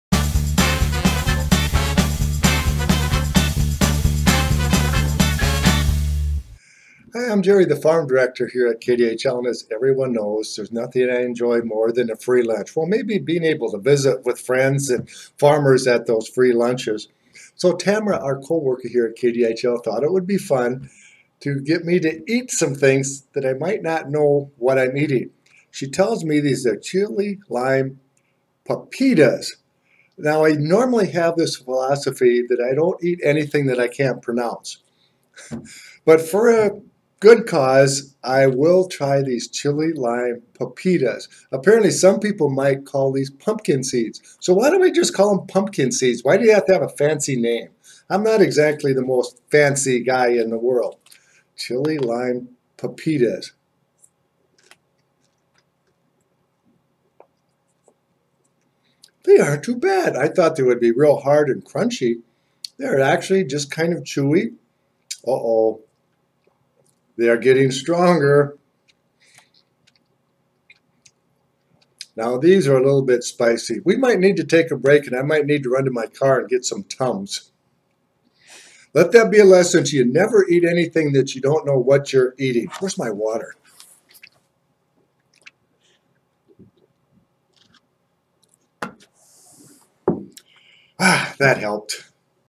Will Jerry Eat It: Spicy Chili Lime Pepitas
KDHL Farm Director Jerry Groskreutz’s day heats up after he tastes spicy chili lime pepitas. Will he feel the burn?